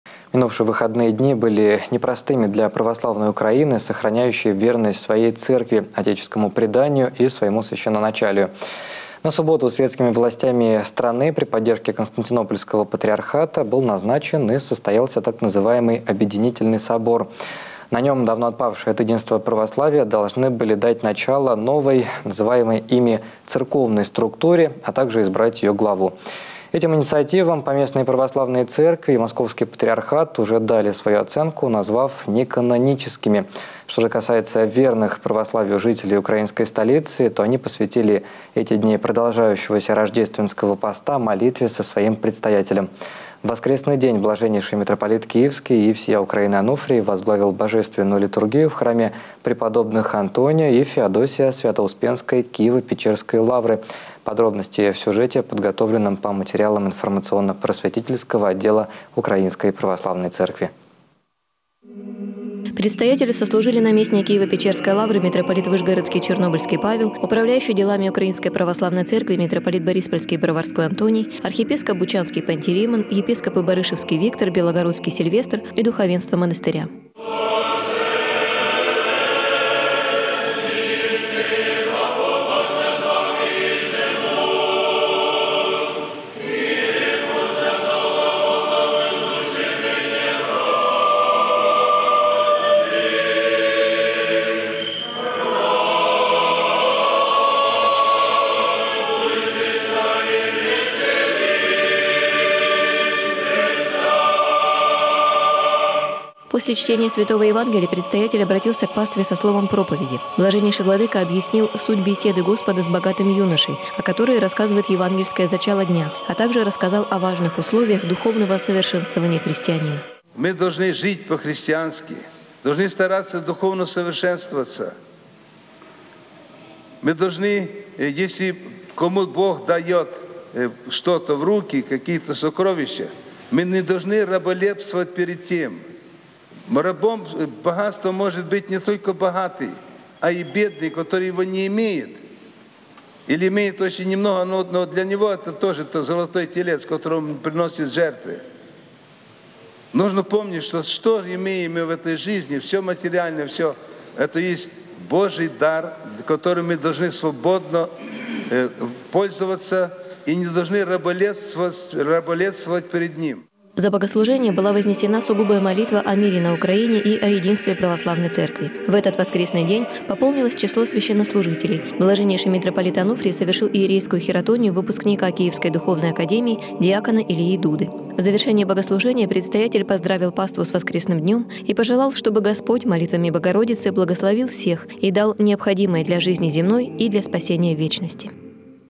Митрополит Онуфрий возглавил Божественную литургию в Свято-Успенской Киево-Печерской Лавре
Минувшие выходные дни были непростыми для православной Украины, сохраняющей верность своей Церкви, отеческому преданию и своему священноначалию. Как известно, на субботу светскими властями страны при поддержке Константинопольского Патриархата был назначен, и состоялся, так называемый Объединительный собор. На нем давно отпавшие от единства Православия раскольники должны были дать начало некой новой, называемой ими «церковной», структуре, а также избрать ее главу. Этим инициативам Поместные Православные Церкви и, разумеется, Московский Патриархат, уже дали свою оценку, назвав неканоническими. Что же касается верных Православию жителей Украинской столицы, то они посвятили эти дни продолжающегося Рождественского поста молитве со своим законным Предстоятелем. В воскресный день Блаженнейший Митрополит Киевский и всея Украины Онуфрий возглавил Божественную литургию в храме преподобных Антония и Феодосия Свято-Успенской Киево-Печерской Лавры.